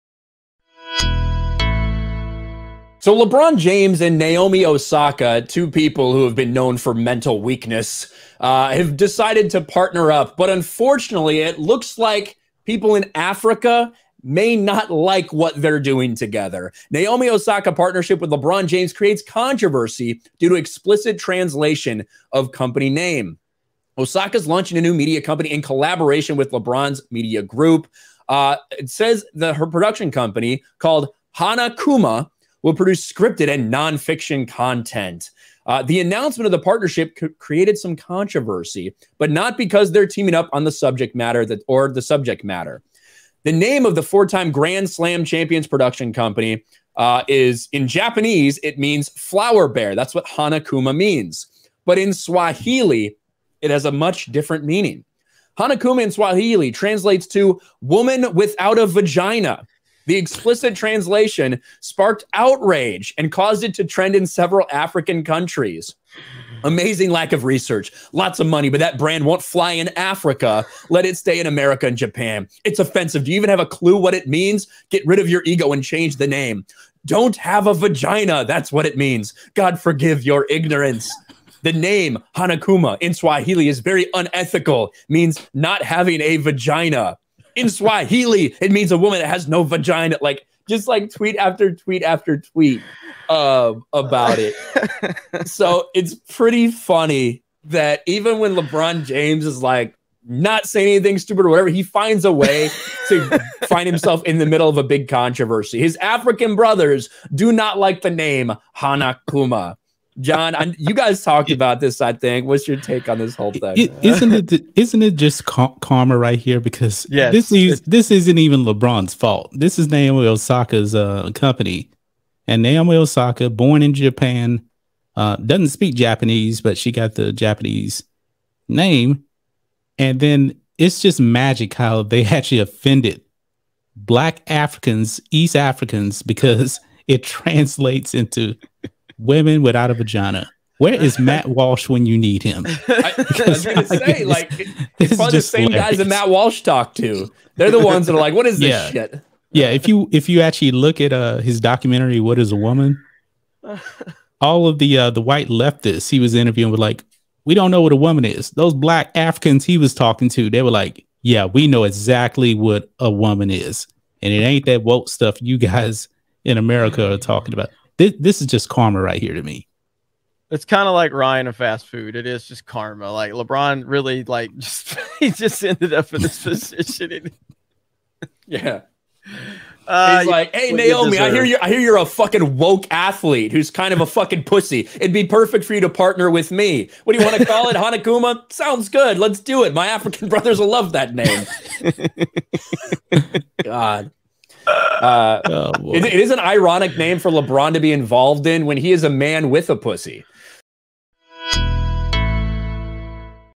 Lebron James And Naomi Osaka Get BLASTED Over Embarrassing Name Of New Company | Africa HATES It!
Jeremy Prime
Pensacola
Florida, USA
32524